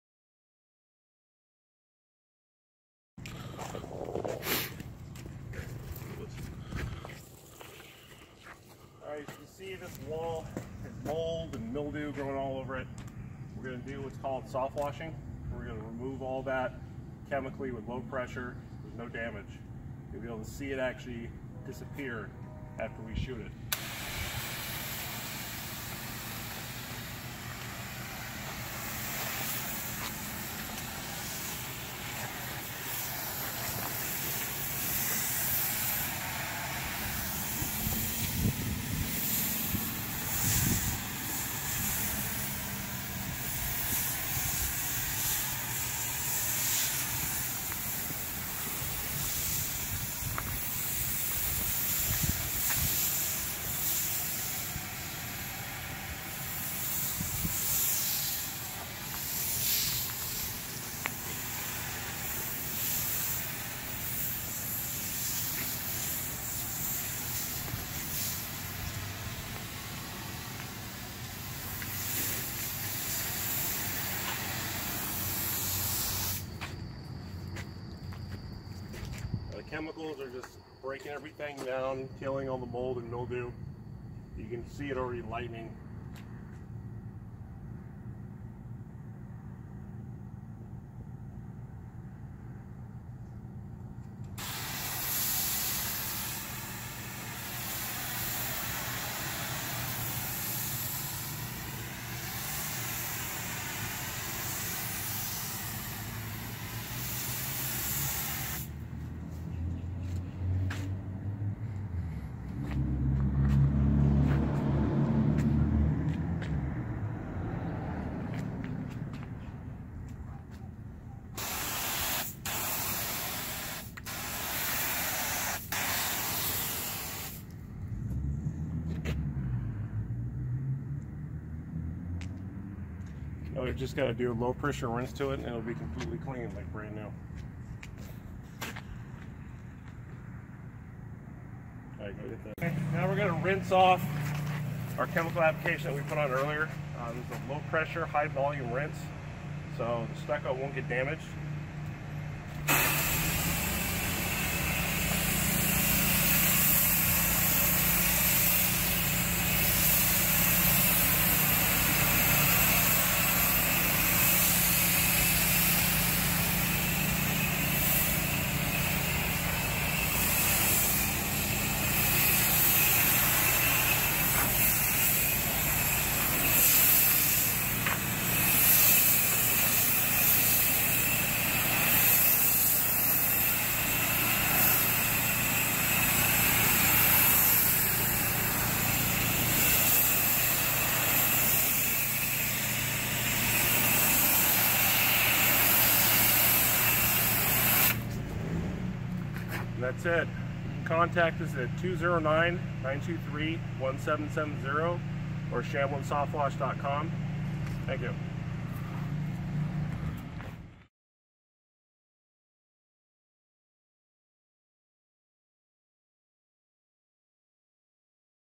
Shamblin Softwash shows our process.
Here we are softwashing stucco to remove mold, mildew, algae, moss, and/or fungus without damaging the delicate surface. We cleaned this wall with Shamblin Softwash Sauce, a multi purpose soap/surfactant  used in softwashing and pressure washing. Shamblin Softwash Sauce is available at purewaterpower.com